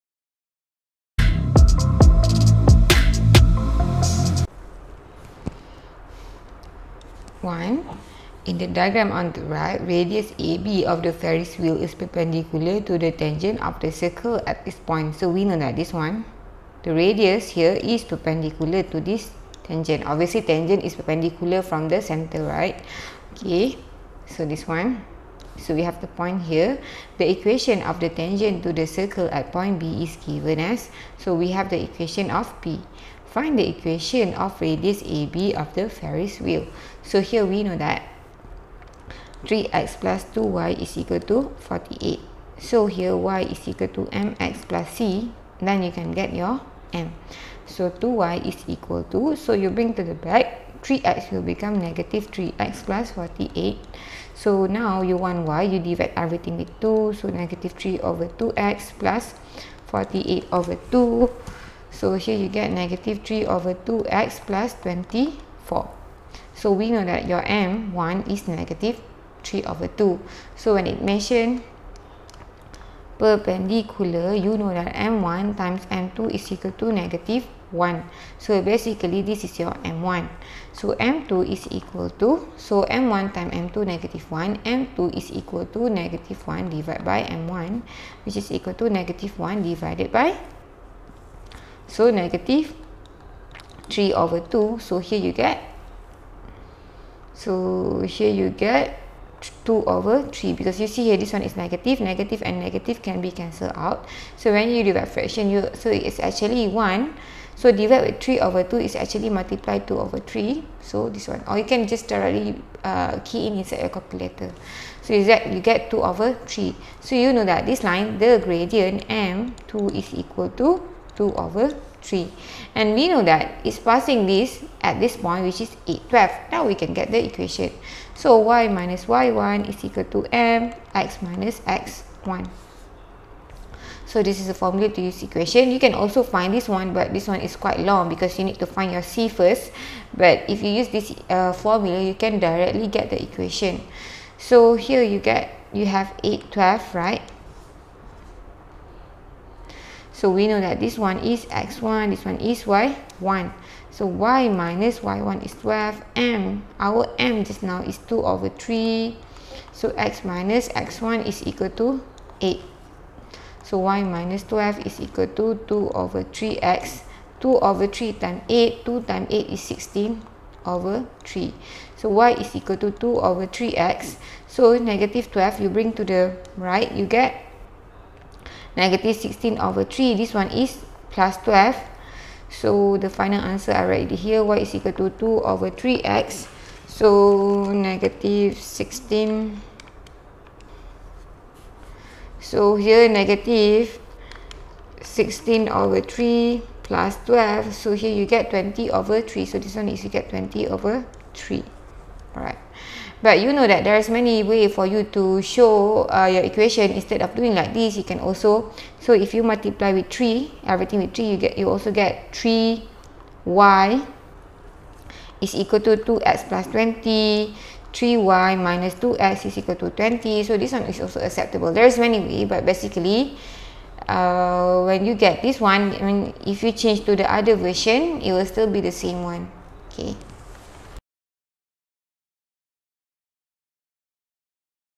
Coordinate geometry self practice 7.5 Q1 latih diri 7.5 geometri koordinat add maths tingkatan 4
Coordinate geometry self practice 7.5 Q1 latih diri 7.5 matematik tambahan geometri koordinat add maths tingkatan 4

In the diagram on the right, radius AB of the Ferris wheel is
 perpendicular to the tangent of the circle at point B(8, 12).
The equation of the tangent to the circle at point B is given
as 3x + 2y = 48. Find the equation of radius AB of the
 Ferris wheel.